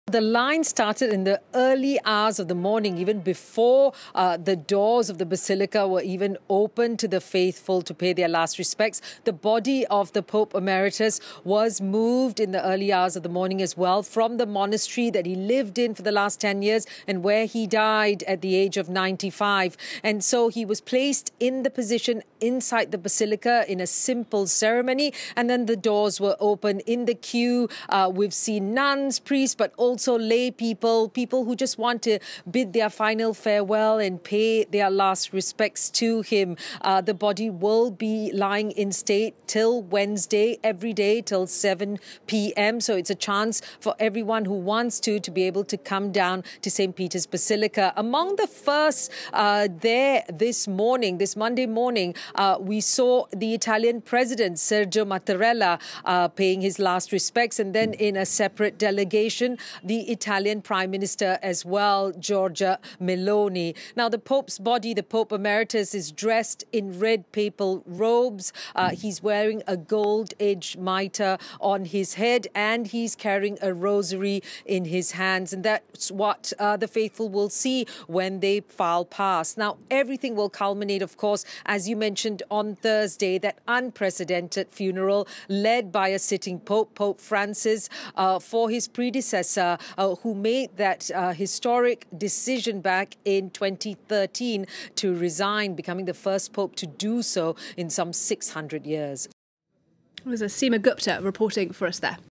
Tens of thousands view Benedict XVI's body at Vatican • FRANCE 24 English
Pope Emeritus Benedict XVI 's body, his head resting on a pair of crimson pillows, lay in state in St. Peter's Basilica on Monday as tens of thousands of people filed by to pay tribute to the pontiff who shocked the world by retiring a decade ago. FRANCE 24's correspondent in Rome Seema Gupta tells us more.